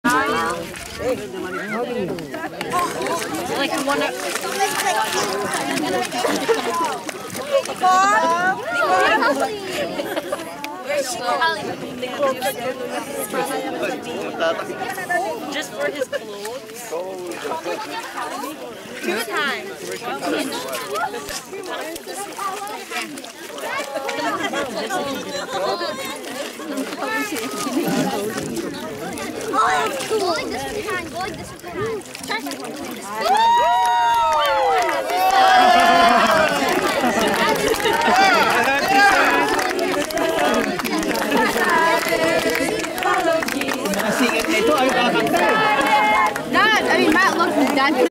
Family Day 2013- Baptism made simple
20 July 2013 - Baptism Rites
Heart of Worship International Church
Bird's Hill Provincial Park, Wpg, Canada
S580-